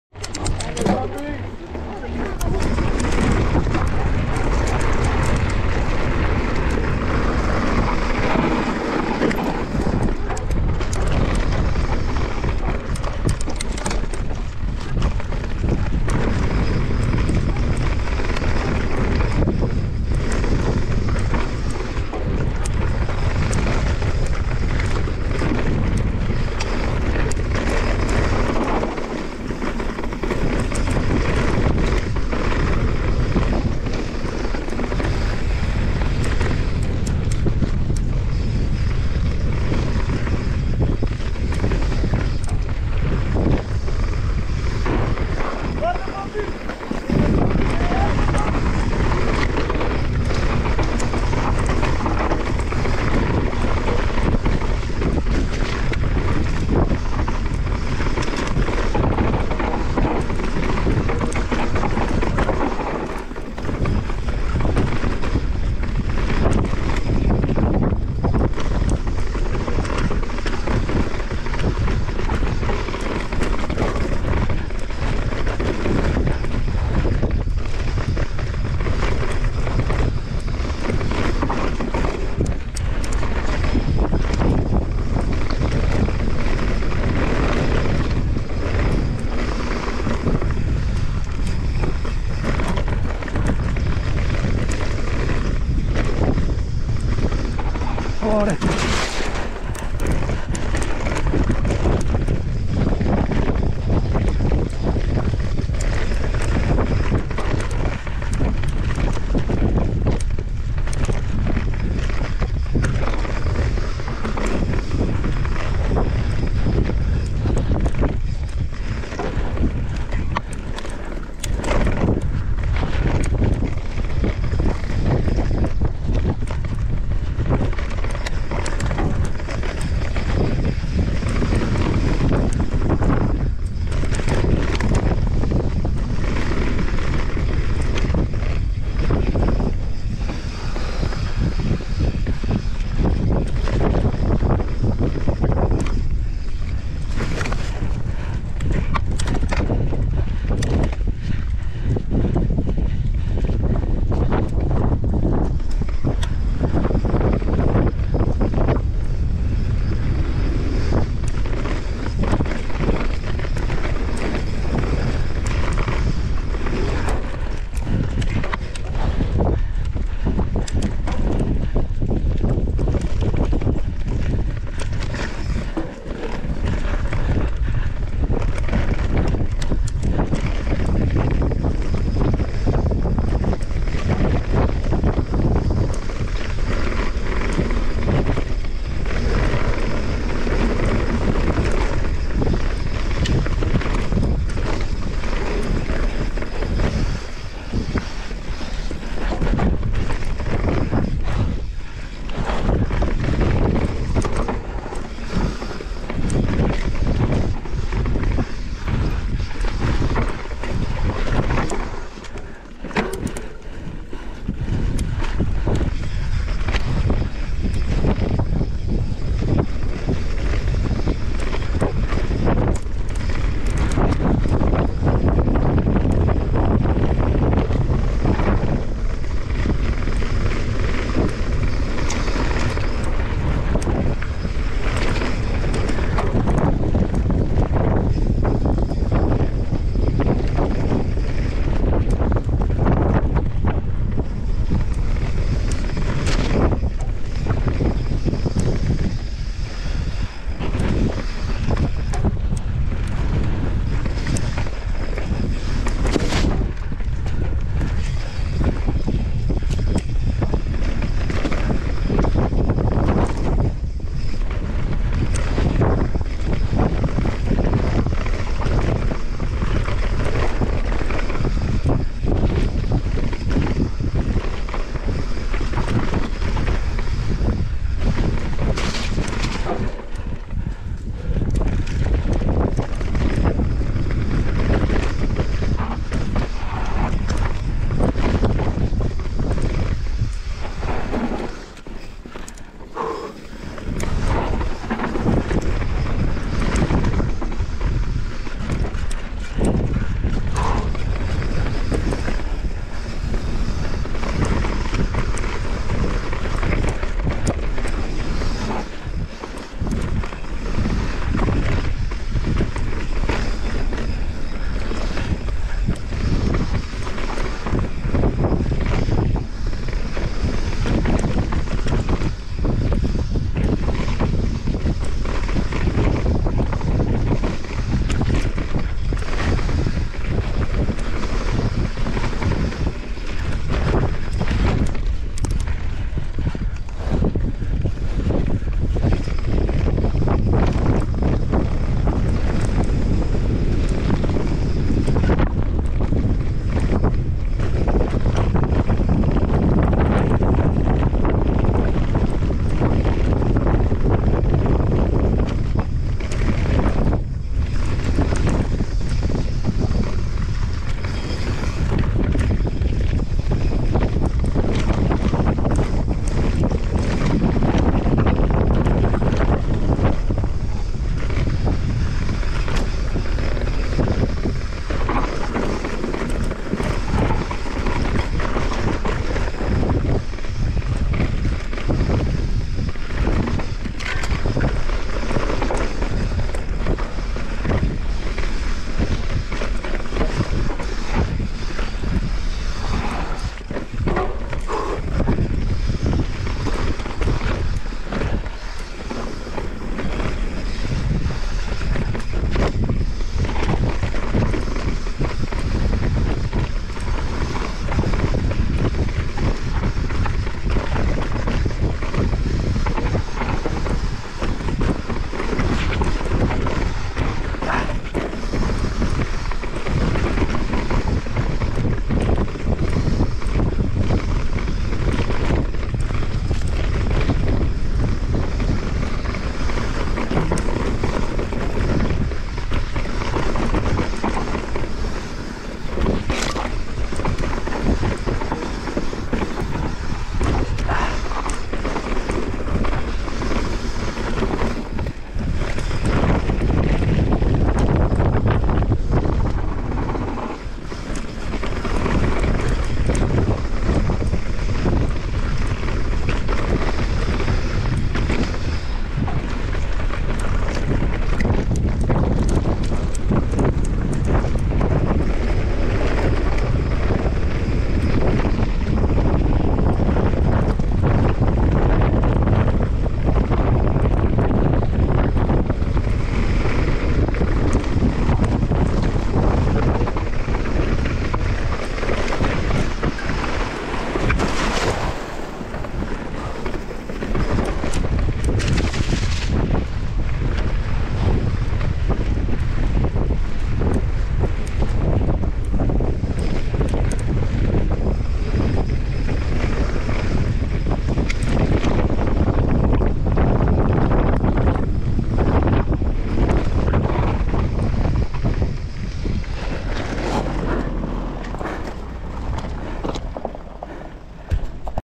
Anilio park Chamois enduro mtb race stage 3 - the mega trail 3.5km 450m descent
same trail from last year but different bike
santacruz megatower cc 2023